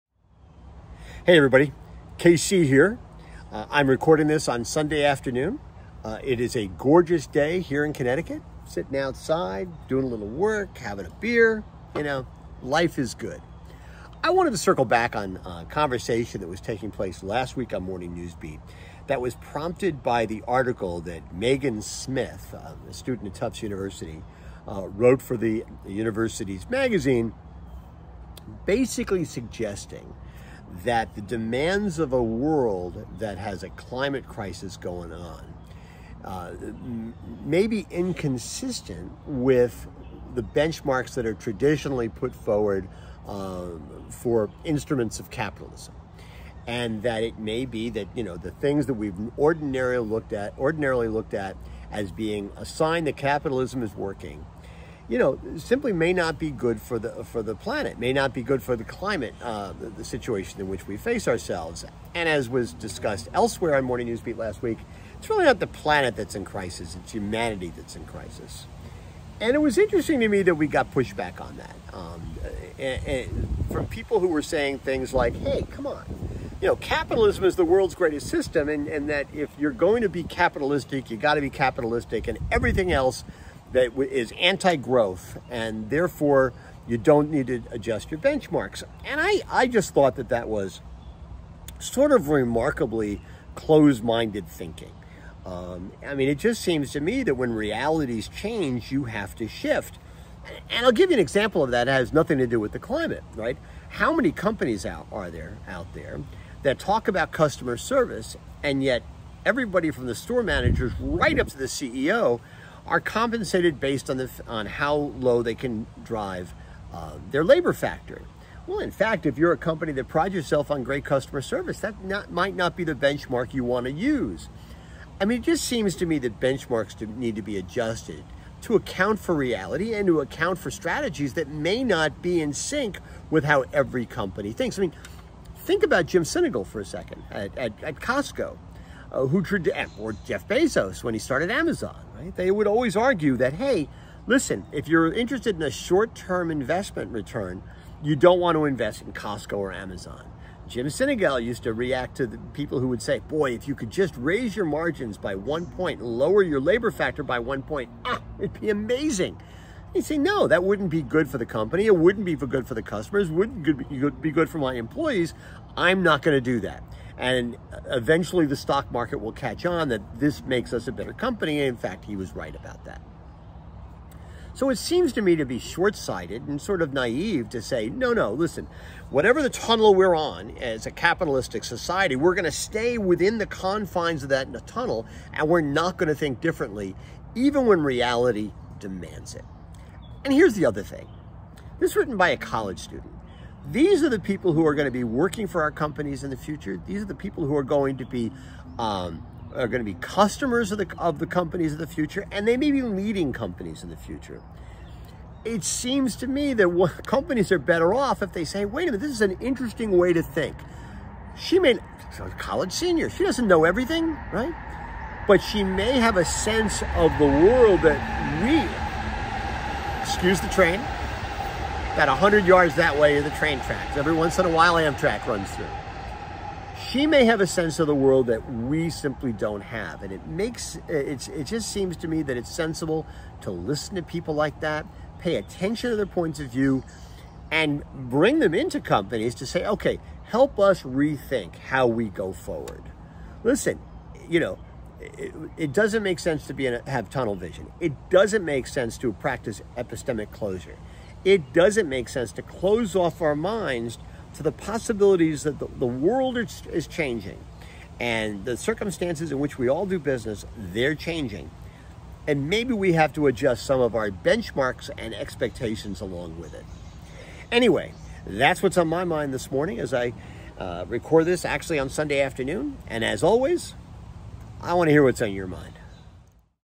06 06 22 FaceTime with the Content Guy
"Matters of Degree"

Today, I return to a conversation we were having last week here on MNB, prompted by an essay written by Meghan Smith, a Tufts University graduating senior who had the temerity - to some folks, at least - to suggest that issues of climate change require that the traditional benchmarks of capitalism need to be adjusted to some degree.  (Pun mine.  Totally intended.). Frankly, this didn't strike me as all that radical - to me, the mark of a great leader is being able to recognize when the winds are shifting, and adjust to these changes and still move forward.  The mark of a lousy leader, I'd suggest, is ignoring reality because it doesn't comport with your biases and preferences.